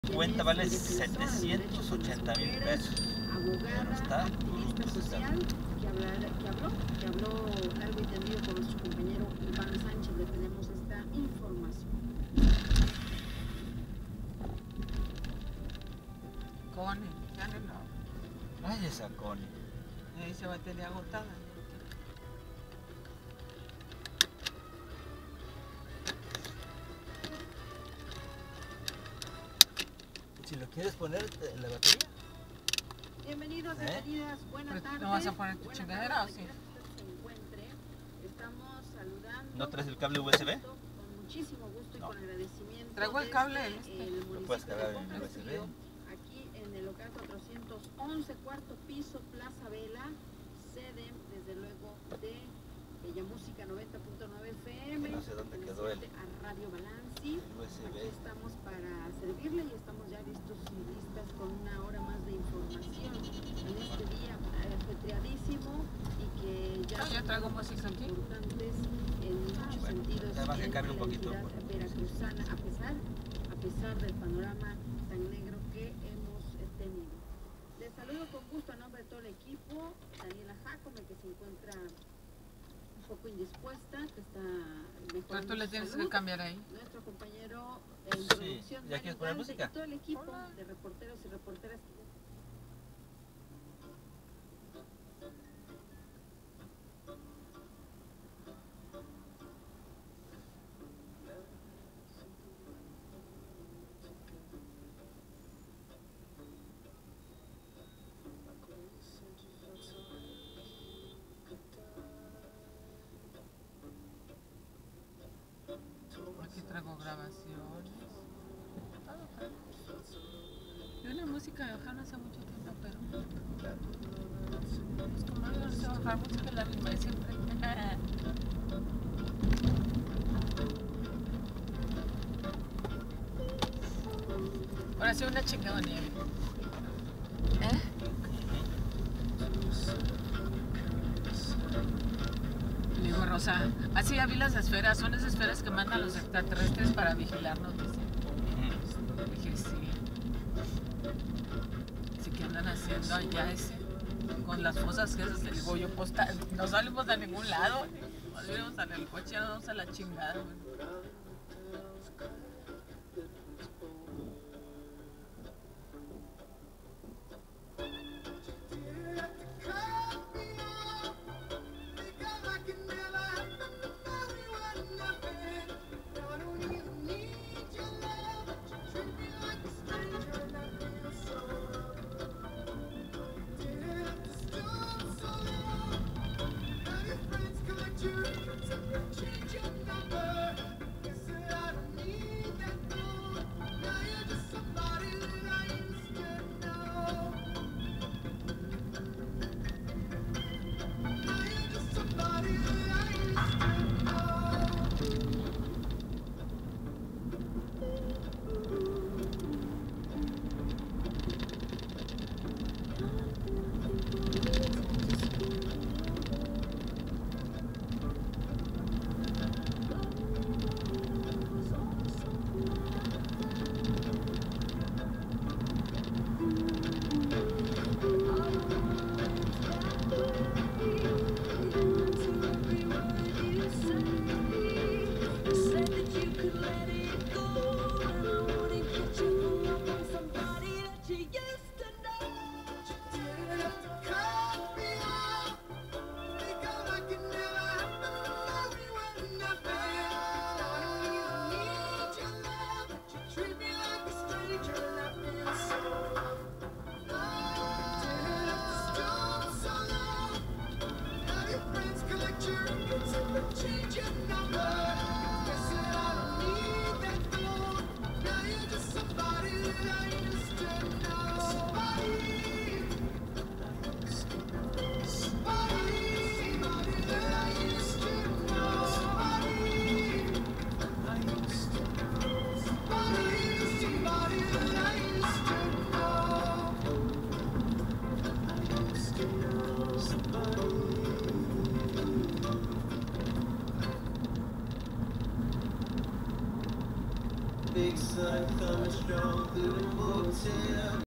Paseando por Veracruz puerto 3
En auto Mini Cooper S modelo 2016